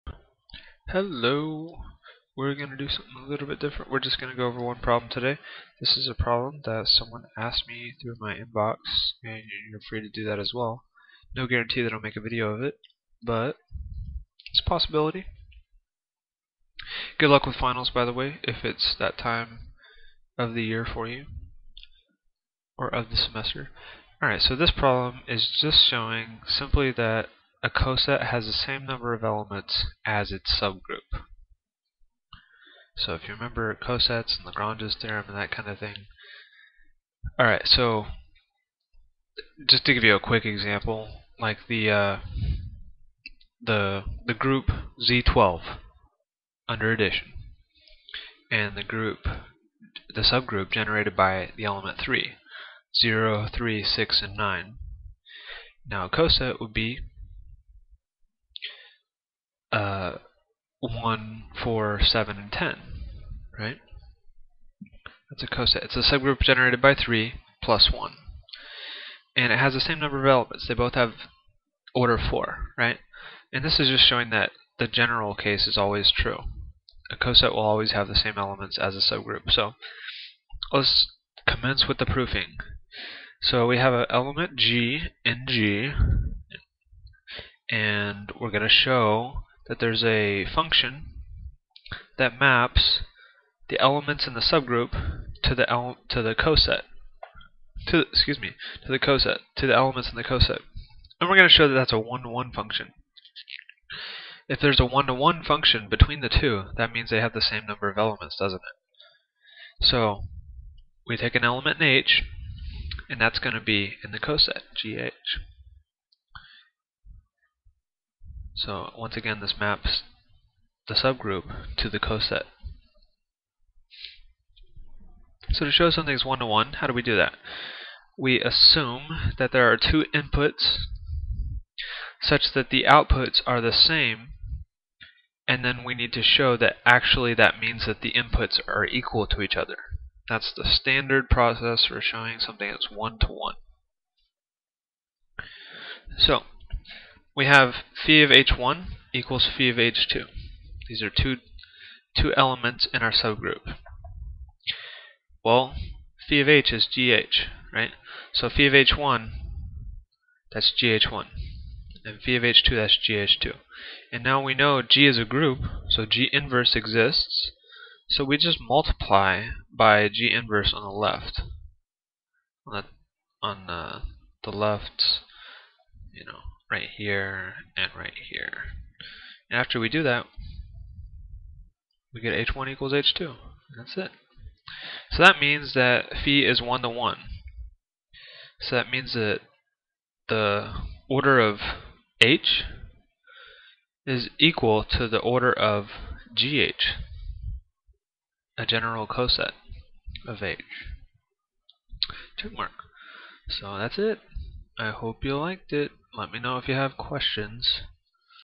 Modern Algebra AKA Abstract Algebra) Made Easy Part 6_2 Cosets & Lagrange's Theorem
Just a quick proof from a question someone asked me~